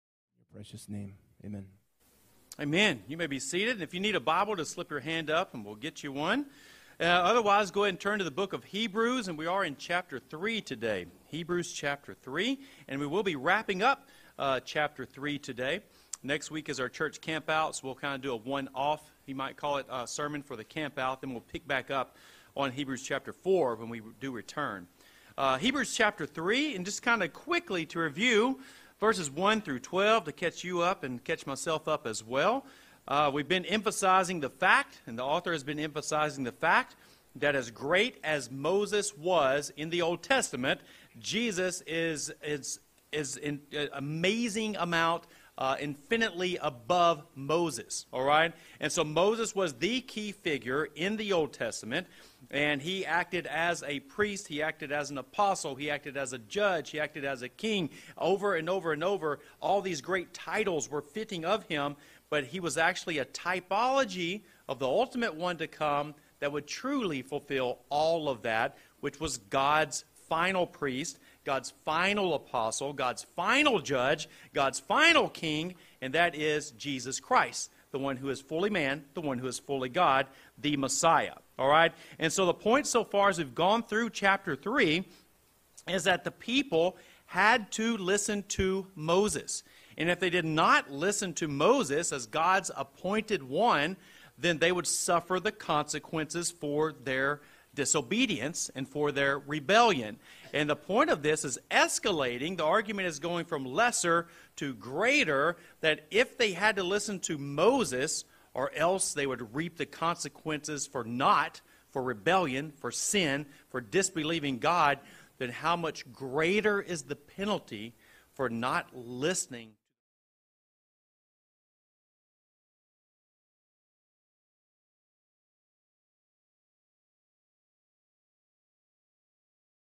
Hebrews 3:12-19 - Trey Talley
The author of Hebrews labors the point of realizing who these people were that provoked God, sinned against God, and died without reaching the Promised Land. They were the people who we would think to be the least likely to rebel against God. So, it is with the people that the author of Hebrews is writing. What about you, are you with the people of God, but not a person of God? Follow the advice of the author of Hebrews:
"Take care, brothers, lest there be in any of you an evil, unbelieving heart, leading you to fall away from the living God. But exhort one another every day, as long as it is called "today," that none of you may be hardened by the deceitfulness of sin." - Hebrews 3:12-13